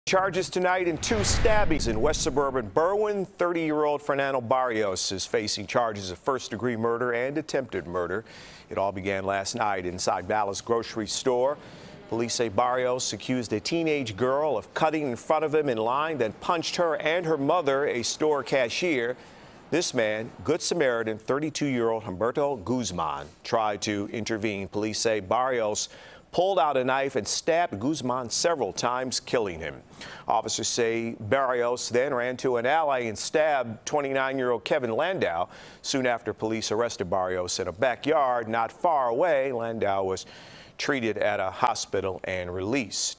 Man Charged In Stabbing Death Of Good Samaritan Who Tried To Stop Attack On Teen
A Berwyn man has been charged in the stabbing death of Humberto Guzman, a good Samaritan who tried to stop an attack on a teen in a Berwyn grocery store Friday.